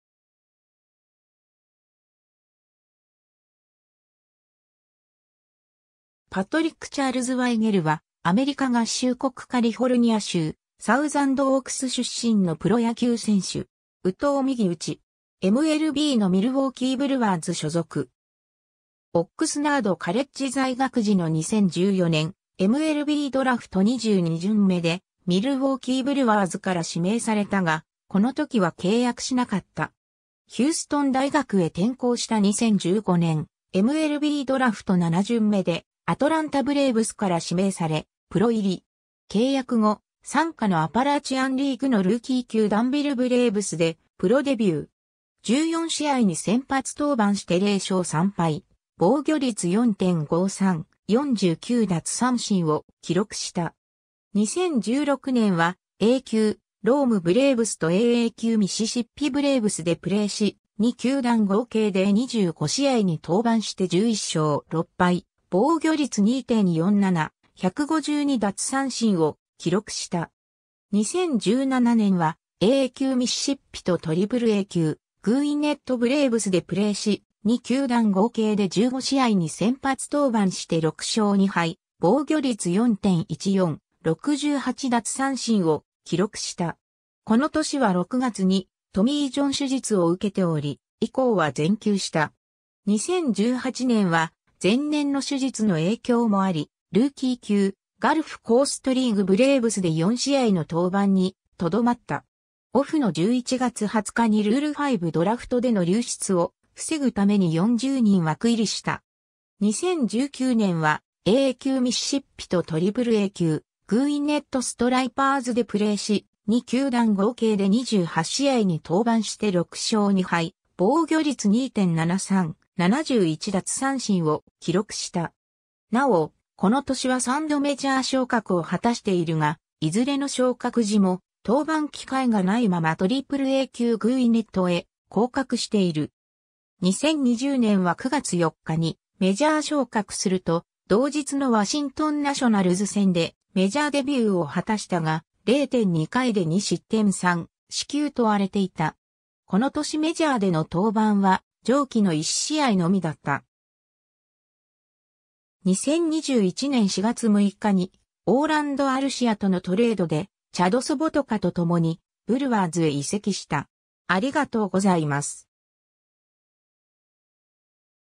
パトリック・ワイゲル
パトリック・チャールズ・ワイゲル（Patrick Charles Weigel, 1994年7月8日 - ）は、 アメリカ合衆国カリフォルニア州サウザンドオークス出身のプロ野球選手（投手）。
右投右打。
MLBのミルウォーキー・ブルワーズ所属。
 オックスナード・カレッジ在学時の2014年、MLBドラフト22巡目（全体656位）でミルウォーキー・ブルワーズから指名されたが、この時は契約しなかった。
 ヒューストン大学へ転校した2015年、MLBドラフト7巡目（全体210位）でアトランタ・ブレーブスから指名され、プロ入り。
契約後、傘下のアパラチアンリーグのルーキー級ダンビル・ブレーブスでプロデビュー。
14試合に先発登板して0勝3敗、防御率4.53、49奪三振を記録した。
 2016年はA級ローム・ブレーブスとAA級ミシシッピ・ブレーブスでプレーし、2球団合計で25試合（先発24試合）に登板して11勝6敗、防御率2.47、152奪三振を記録した。
 2017年はAA級ミシシッピとAAA級グウィネット・ブレーブスでプレーし、2球団合計で15試合に先発登板して6勝2敗、防御率4.14、68奪三振を記録した。
この年は6月にトミー・ジョン手術を受けており、以降は全休した。
 2018年は前年の手術の影響もあり、ルーキー級ガルフ・コーストリーグ・ブレーブスで4試合の登板にとどまった。
 2019年はAA級ミシシッピとAAA級グウィネット・ストライパーズでプレーし、2球団合計で28試合（先発18試合）に登板して6勝2敗、防御率2.73、71奪三振を記録した。
なお、この年は3度メジャー昇格を果たしているが、いずれの昇格時も登板機会が無いままAAA級グウィネットへ降格している。
 2020年は9月4日にメジャー昇格すると、同日のワシントン・ナショナルズ戦でメジャーデビューを果たしたが、0.2回で2失点3四球と荒れていた。
この年メジャーでの登板は上記の1試合のみだった。
 2021年4月6日にオーランド・アルシアとのトレードで、チャド・ソボトカと共にブルワーズへ移籍した。